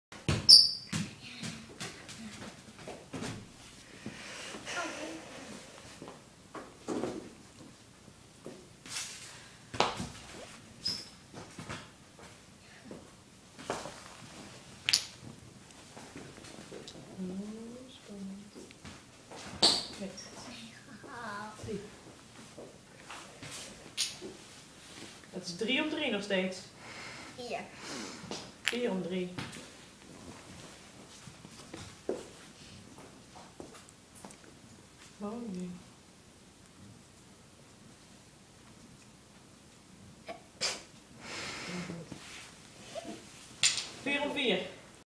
Fysiotherapie Coco Huiberts nr 3
Coco heeft spierziekte en heeft 1x in de week fysiotherapie